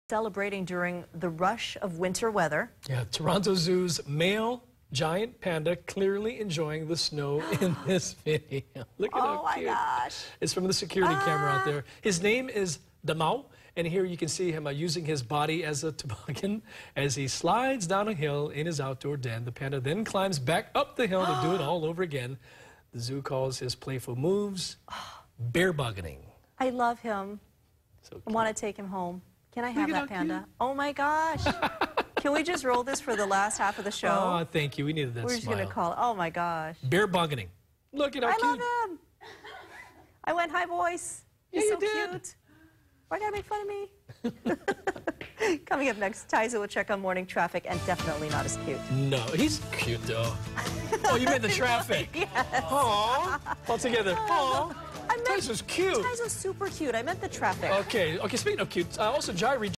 Watch: Giant Panda tumbles in the snow
The Toronto zoo's male giant panda is celebrating this rush of winter weather.